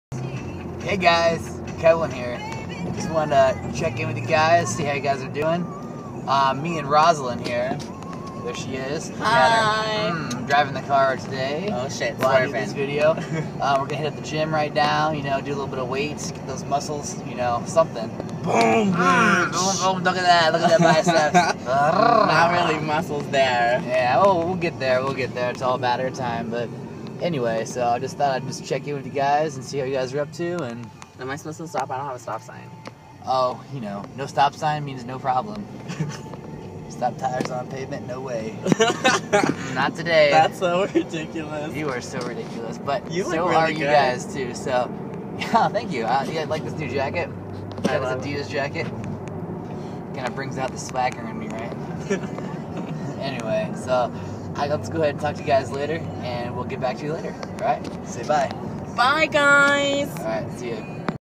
Kevlyn Heads To The Gym Pt. 1
Roselyn heads to the gym with Kevin to get a good work out!